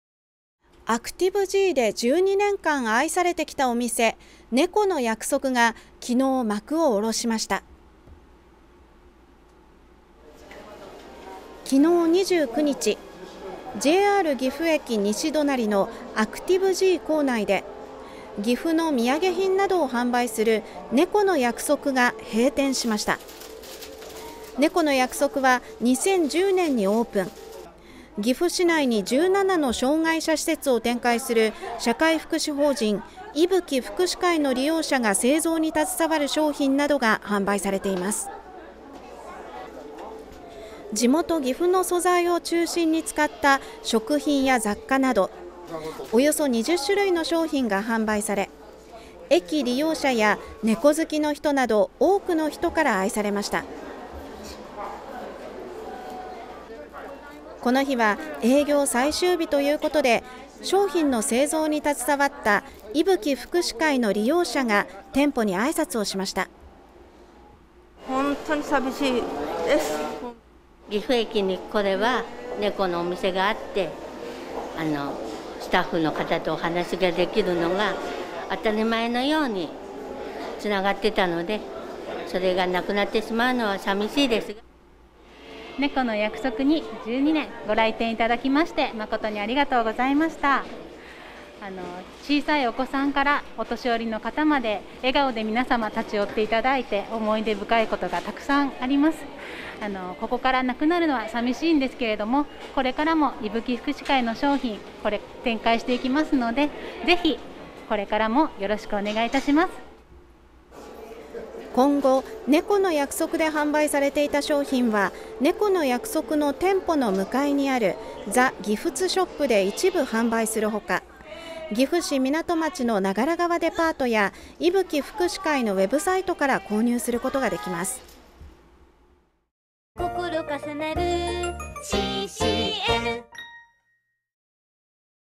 【岐阜のニュース】ねこの約束 閉店（2022年3月30日放送）
岐阜県のケーブルテレビＣＣＮ（シーシーエヌ）の番組やＣＭ・ニュースを公開！

ＪＲ岐阜駅のアクティブＧで１２年間愛されてきたお店「ねこの約束」が幕を下ろしました。